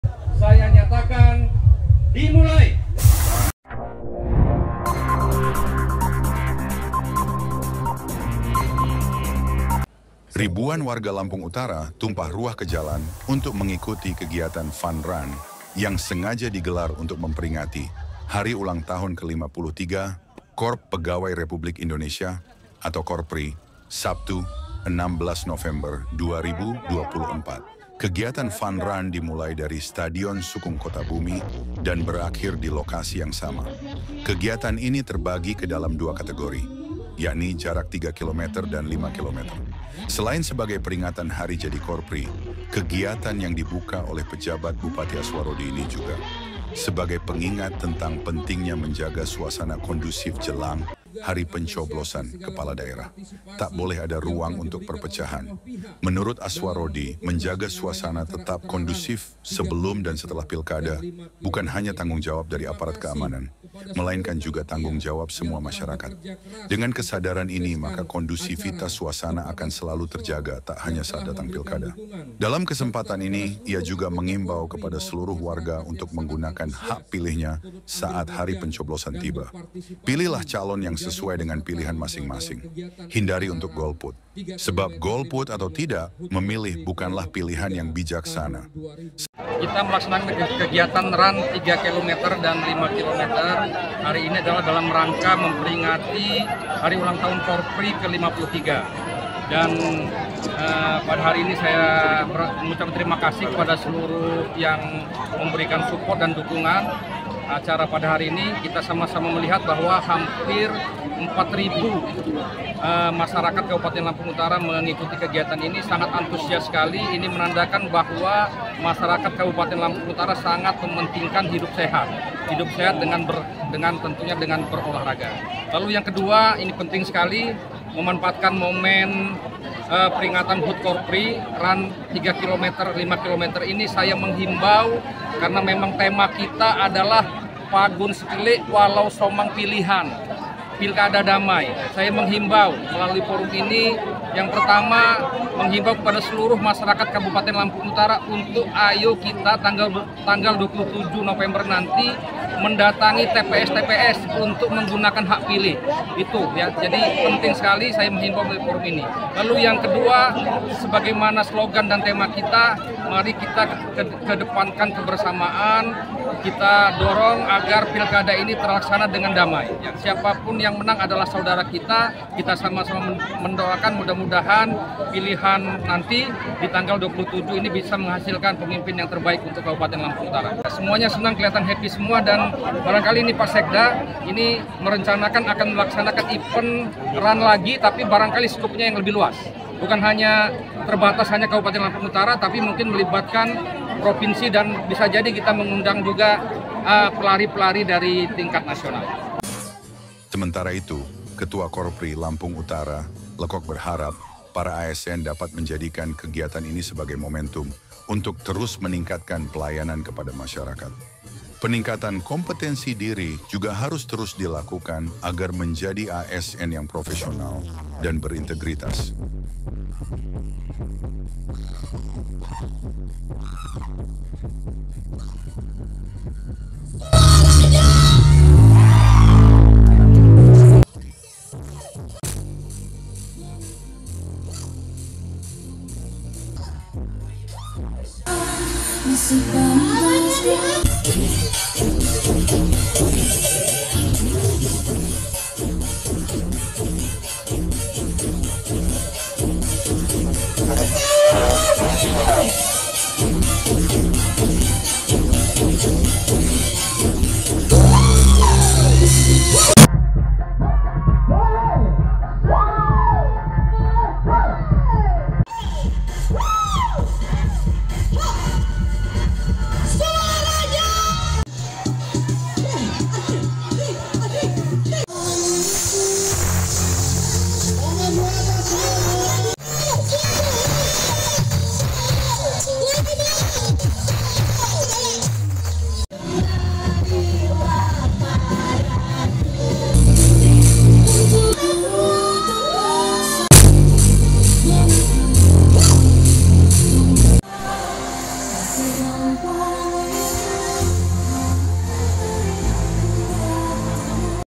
Ribuan Warga Ramaikan Fun Run Hut Ke-53 Korpri
Ribuan warga Lampung Utara tumpah ruah ke jalan untuk mengikuti kegiatan fun run yang sengaja digelar untuk memperingati hari ulang tahun ke-53 Korps Pegawai Republik Indonesia atau Korpri, Sabtu, 16 November 2024.

Kegiatan fun run dimulai dari Stadion Sukung Kotabumi dan berakhir di lokasi yang sama. Kegiatan ini terbagi ke dalam dua kategori, yakni jarak 3 Kilometer dan 5 Kilometer.

Selain sebagai peringatan hari jadi Korpri, kegiatan yang dibuka oleh Pejabat Bupati Aswarodi ini juga sebagai pengingat tentang pentingnya menjaga suasana kondusif jelang hari pencoblosan kepala daerah. Tak boleh ada ruang untuk perpecahan.

Menurut Aswarodi, menjaga suasana tetap kondusif sebelum dan setelah Pilkada bukan hanya tanggung jawab dari aparat keamanan, melainkan juga tanggung jawab semua masyarakat. Dengan kesadaran ini maka kondusivitas suasana akan selalu terjaga tak hanya saat datang Pilkada.

Dalam kesempatan ini, ia juga mengimbau kepada seluruh warga untuk menggunakan hak pilihnya saat hari pencoblosan tiba. Pilihlah calon yang sesuai dengan pilihan masing masing. Hindari untuk golput. Sebab, golput atau tidak memilih bukanlah pilihan yang bijaksana.

Sementara itu, Ketua Korpri Lampung Utara, Lekok berharap, para ASN dapat menjadikan kegiatan ini sebagai momentum untuk terus meningkatkan pelayanan kepada masyarakat. Peningkatan kompetensi diri juga harus terus dilakukan agar menjadi ASN yang profesional dan berintegritas.